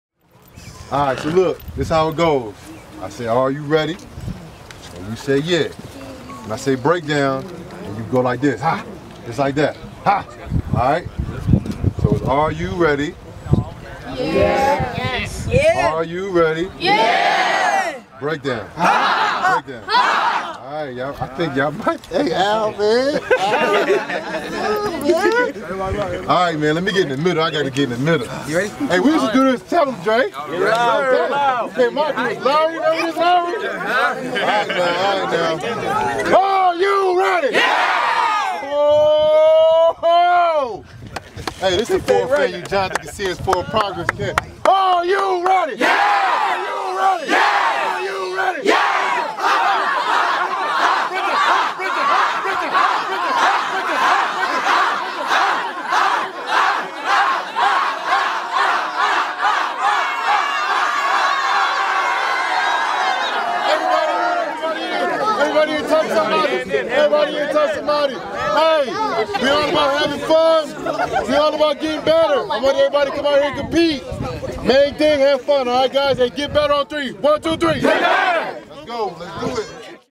Giants' Jonathan Casillas gets kids excited
New York Giants linebacker Jonathan Casillas whips his campers into a frenzy at the start of his 2017 Forward Progress Football and Life Skills Camp at New Brunswick Middle School. 6/17/2017  (Andrew Mills | NJ Advance Media)